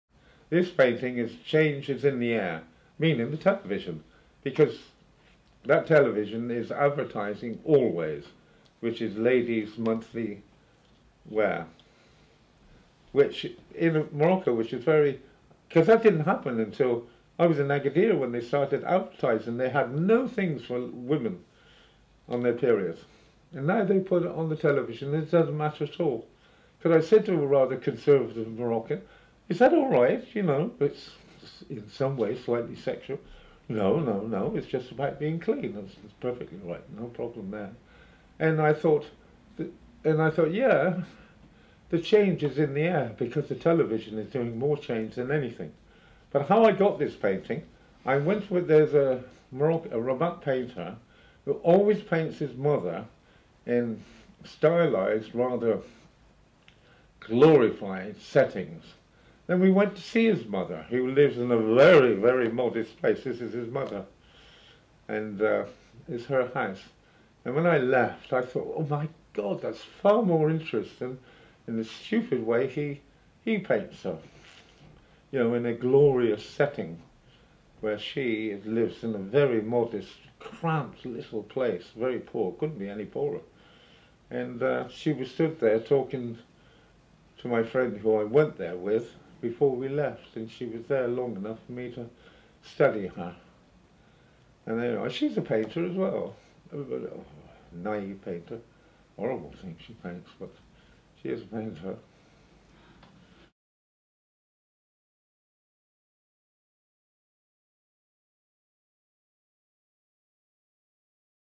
Roger Davis - 2000 Change is in the Air, Rabat, Morocco Painting
2000 Change is in the Air, Rabat, Morocco - A Painting of an interior Moroccan scene - 120cm x 120cm - For more information please